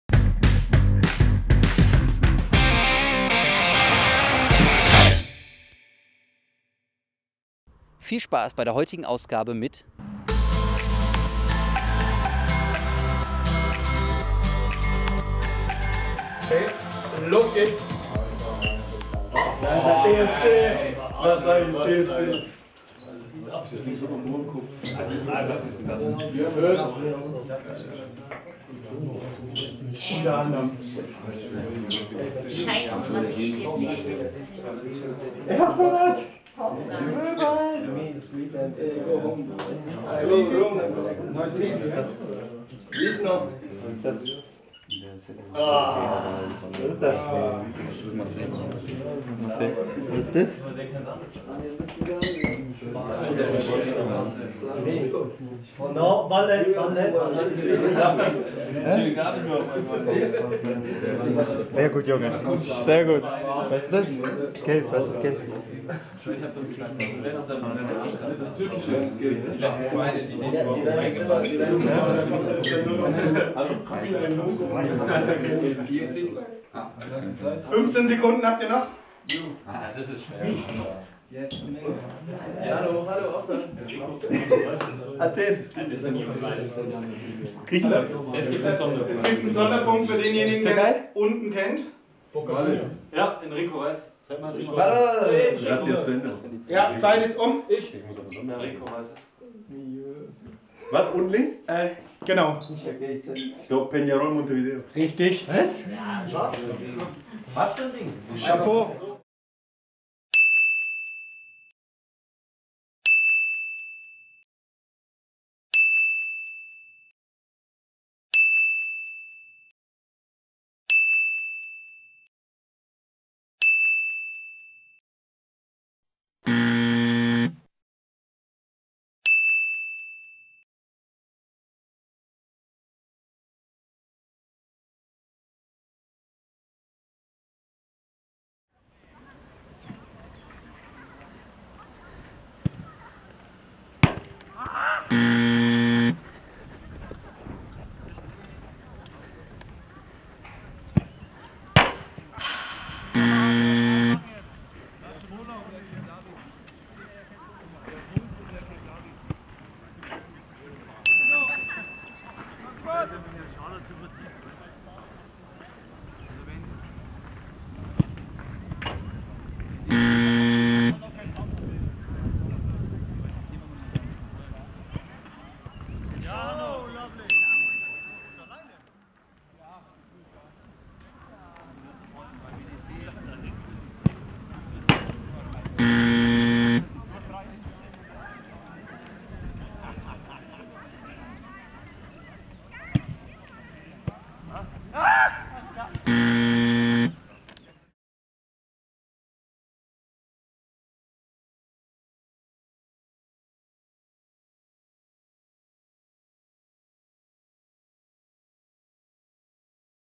"Kennen und Können" mit Behrens und Rhein
Alle Jahre wieder steht das Zimmerduell im Trainingslager auf dem Programm. In diesem Jahr müssen die Spieler in zwei Teilen ihr Wissen und ihr Können unter Beweis stellen.
Im sechsten Teil stellen sich Hanno Behrens und Simon Rhein den Aufgaben.